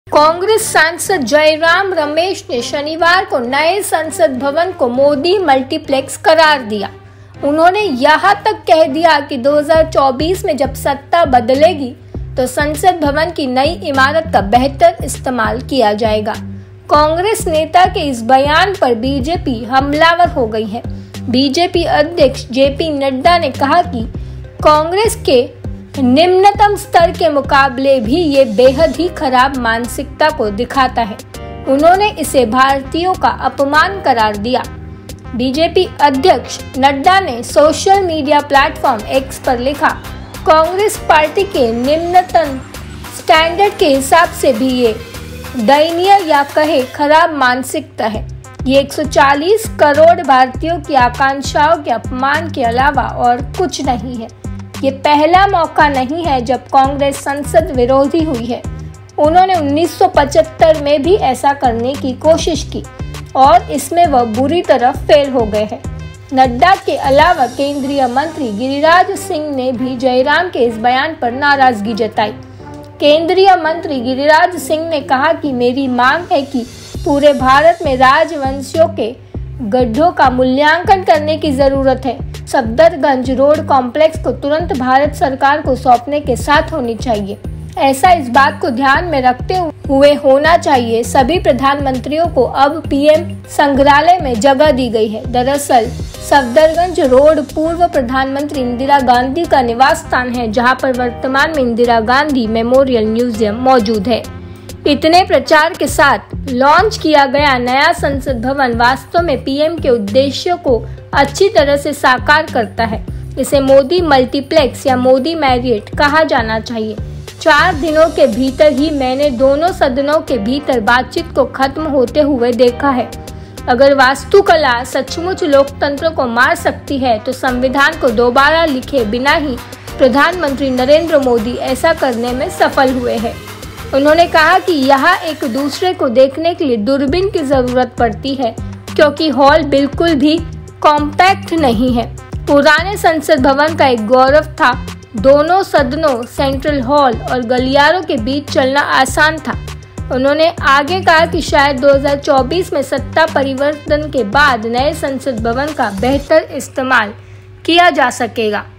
Congress सांसद जयराम रमेश ने नए संसद भवन पर कुछ ऐसा कह दिया , जिसकी वजह से बीजेपी उन पर हमलावर हो गई
कांग्रेस सांसद जयराम रमेश ने शनिवार को नए संसद भवन को 'मोदी मल्टीप्लेक्स' करार दिया. उन्होंने यहां तक कह दिया कि 2024 में जब सत्ता बदलेगी, तो संसद भवन की नई इमारत का बेहतर इस्तेमाल किया जाएगा. कांग्रेस नेता के इस बयान पर बीजेपी हमलावर हो गई है. बीजेपी अध्यक्ष जेपी नड्डा ने कहा कि कांग्रेस के निम्नतम स्तर के मुकाबले भी ये बेहद ही खराब मानसिकता को दिखाता है. उन्होंने इसे भारतीयों का अपमान करार दिया. बीजेपी अध्यक्ष नड्डा ने सोशल मीडिया प्लेटफॉर्म 'एक्स' पर लिखा, 'कांग्रेस पार्टी के निम्नतम स्टैंडर्ड के हिसाब से भी ये दयनीय या कहें खराब मानसिकता है. ये 140 करोड़ भारतीयों की आकांक्षाओं के अपमान के अलावा और कुछ नहीं है. ये पहला मौका नहीं है, जब कांग्रेस संसद-विरोधी हुई है. उन्होंने 1975 में भी ऐसा करने की कोशिश की और इसमें वह बुरी तरह फेल हो गए.' नड्डा के अलावा केंद्रीय मंत्री गिरिराज सिंह ने भी जयराम के इस बयान पर नाराजगी जताई. केंद्रीय मंत्री गिरिराज सिंह ने कहा कि मेरी मांग है कि पूरे भारत में 'राजवंशों के गढ़ों' का मूल्यांकन करने की जरूरत है. शुरुआत 1, सफरदजंग रोड कॉम्पलैक्स को तुरंत भारत सरकार को सौंपने के साथ होनी चाहिए. ऐसा इस बात को ध्यान में रखते हुए होना चाहिए सभी प्रधानमंत्रियों को अब पीएम संग्रहाल में जगह दी गई है. दरअसल, 1, सफदरजंग रोड पूर्व प्रधानमंत्री इंदिरा गांधी का निवास स्थान है, जहां पर वर्तमान में 'इंदिरा गांधी मेमोरियल म्यूजियम' मौजूद है. इतने प्रचार के साथ लॉन्च किया गया नया संसद भवन वास्तव में पीएम के उद्देश्यों को अच्छी तरह से साकार करता है. इसे मोदी मल्टीप्लेक्स या मोदी मैरियट कहा जाना चाहिए. चार दिनों के भीतर ही मैंने दोनों सदनों के भीतर बातचीत को खत्म होते हुए देखा है. अगर वास्तुकला सचमुच लोकतंत्र को मार सकती है, तो संविधान को दोबारा लिखे बिना ही प्रधानमंत्री नरेंद्र मोदी ऐसा करने में सफल हुए हैं.
उन्होंने कहा कि यहां एक दूसरे को देखने के लिए दूरबीन की जरूरत पड़ती है, क्योंकि हॉल बिल्कुल भी कॉम्पैक्ट नहीं है. पुराने संसद भवन का एक गौरव था. दोनों सदनों, सेंट्रल हॉल और गलियारों के बीच चलना आसान था. उन्होंने आगे कहा कि शायद 2024 में सत्ता परिवर्तन के बाद नए संसद भवन का बेहतर इस्तेमाल किया जा सकेगा.